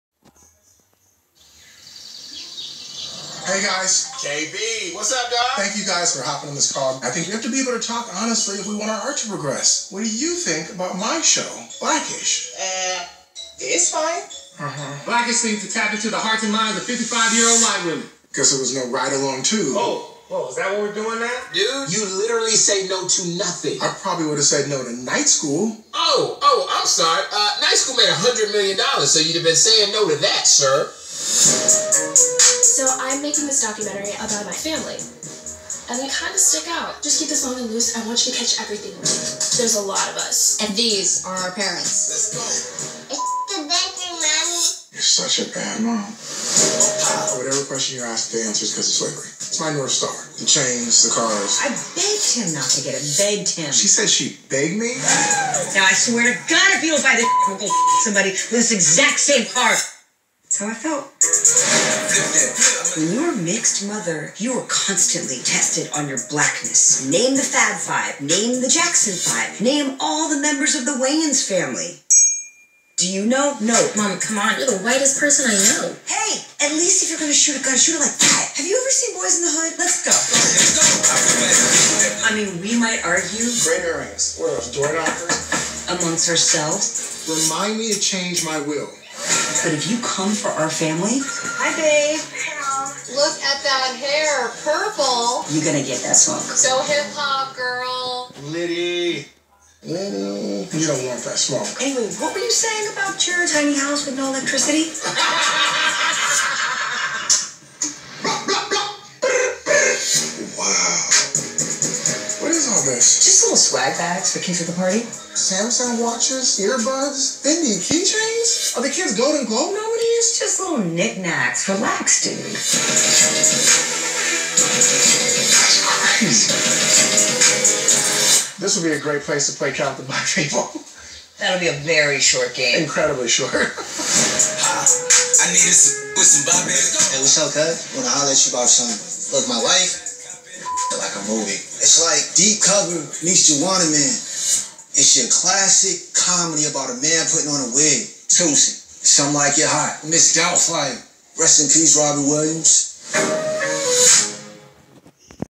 Season 1 Trailer
Kenya Barris and his family navigate relationships, race and culture while grappling with their newfound success in this comedy series.